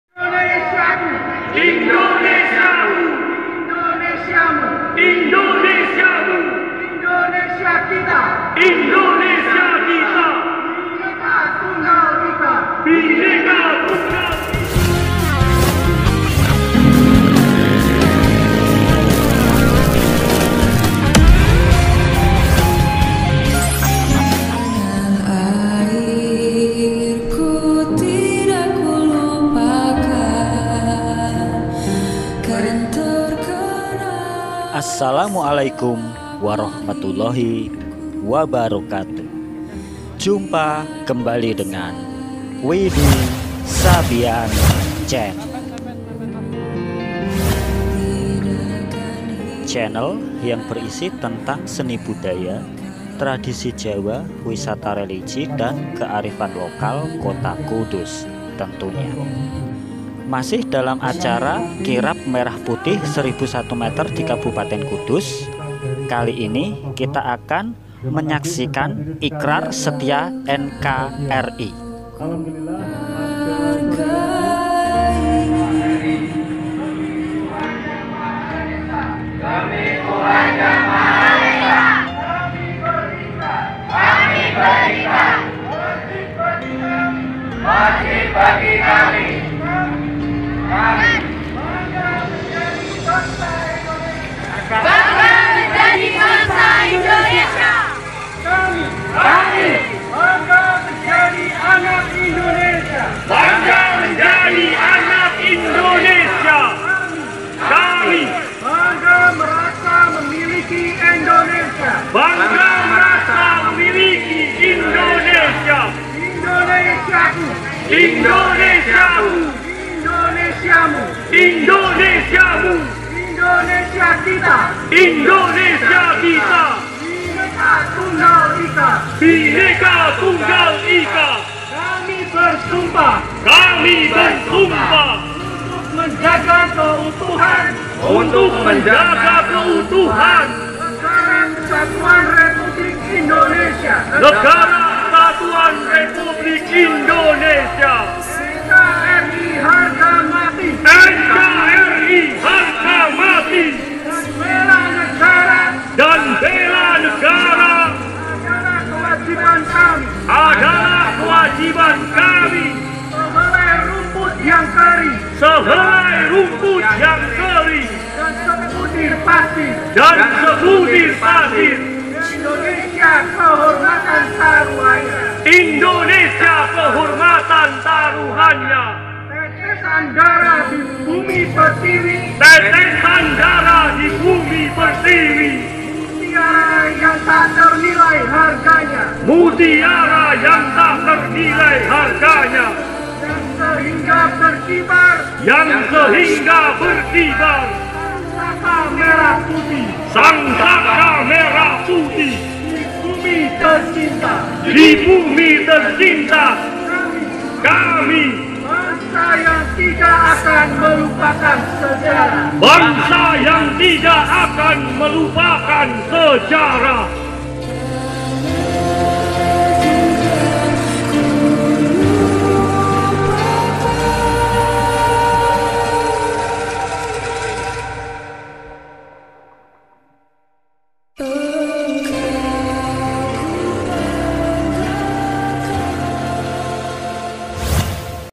Pembacaan Ikrar Setia NKRI Oleh Perwakilan Generasi Muda Lintas Agama Kota KUDUS
pembacaan ikrar setia nkri di bacakan perwakilan generasi muda lintas agama pada puncak acara kirab merah putih di kota kudus minggu 11 desember 2022